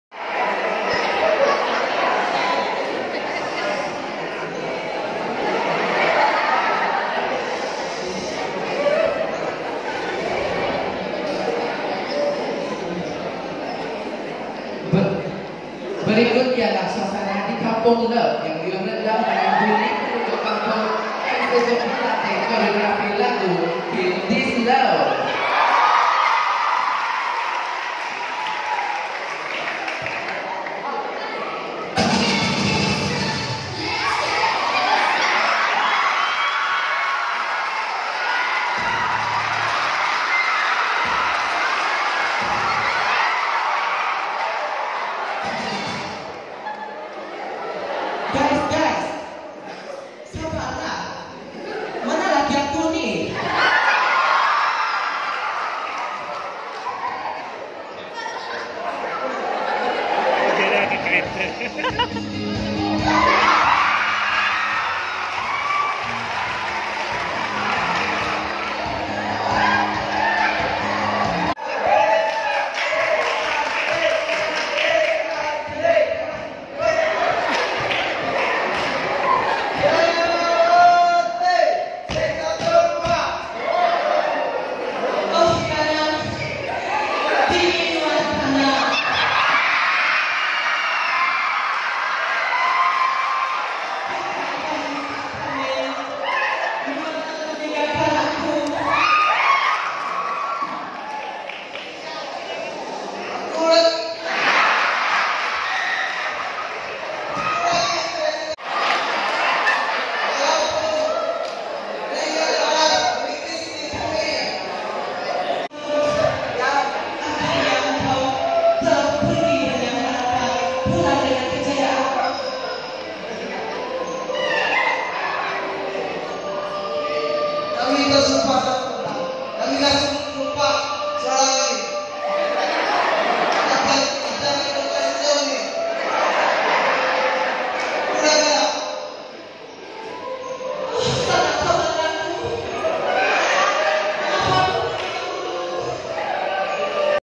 SMK LAKE BAU | Persembahan asrama singgai smk lake sempena ambang merdeka. [Lakers]
Persembahan asrama singgai yang tidak sempurna..ni je yang ada..walaupun cuma clip- clip at least ada kenangan dorang di asrama.